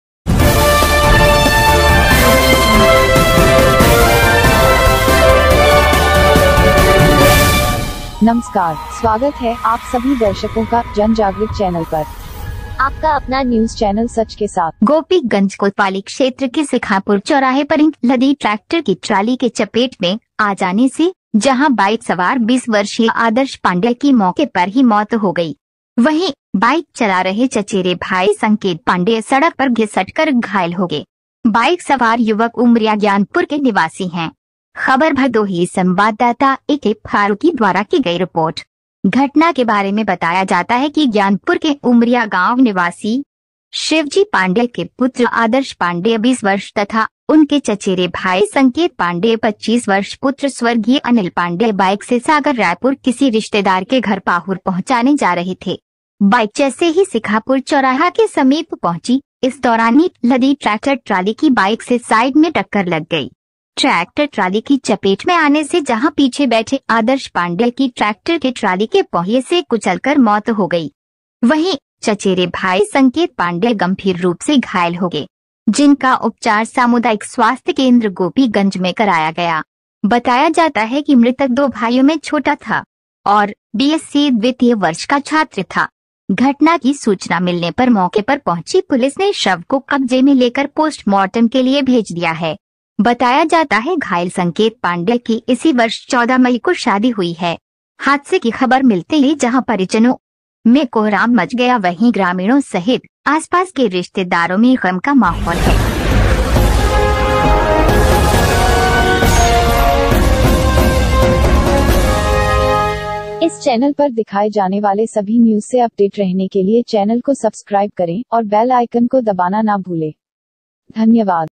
बाईक चला रहे संकेत पांन्डेय सड़क पर घिसटकर घायल हो गये!
गोपी गंज कोतवाली क्षेत्र के सिखापुर चौराहे पर ईंट लदी ट्रैक्टर की ट्राली के चपेट में आ जाने से, जहां बाइक सवार 20 वर्षीय आदर्श पांन्डेय की मौके पर ही मौत हो गई! वहीं,
   बाईक चला रहे चचेरे भाई संकेत पांन्डेय सड़क पर घिसटकर घायल हो गये!
   बाइक सवार युवक उमरिया ज्ञानपुर के निवासी हैं!
  ख़बर भदोही संवाददाता एके फारुकी द्वारा कि गई रिपोर्ट!
   घटना के बारे में बताया जाता है कि ज्ञानपुर के उमरिया गांव निवासी शिवजी पांडेय के पुत्र आदर्श पांडेय 20 वर्ष तथा,
    उनके चचेरे भाई संकेत पांडेय 25 वर्ष पुत्र स्वर्गीय अनिल पांडेय बाइक से सागर रायपुर किसी रिश्तेदार के घर पाहुर पहुंचाने जा रहे थे!
   बाइक जैसे ही सिखापुर चौराहा के समीप पहुंची,  इस दौरान ईट लदी ट्रैक्टर ट्राली की बाइक से साइड में टक्कर लग गई!
    ट्रैक्टर ट्राली की चपेट में आने से जहां पीछे बैठे आदर्श पांडेय की ट्रैक्टर के ट्राली के पहिए से कुचलकर मौत हो गई! वहीं,
   चचेरे भाई संकेत पांडेय गंभीर रुप से घायल हो गये! जिनका उपचार सामुदायिक स्वास्थ्य केंद्र गोपी गंज में कराया गया!
  बताया जाता है कि मृतक दो भाइयों में छोटा था! और, बीएससी द्वितीय वर्ष का छात्र था!
   घटना की सूचना मिलने पर मौके पर पहुंची पुलिस ने शव को कब्जे में लेकर पोस्टमार्टम के लिए भेज दिया है!
   बताया जाता है घायल संकेत पांडेय  की इसी वर्ष 14 मई को शादी हुई है!
   हादसे की खबर मिलते ही जहां परिजनों में कोहराम मच गया वहीं ग्रामीणों सहित,
    आसपास के रिश्तेदारों में ग़म का माहौल है!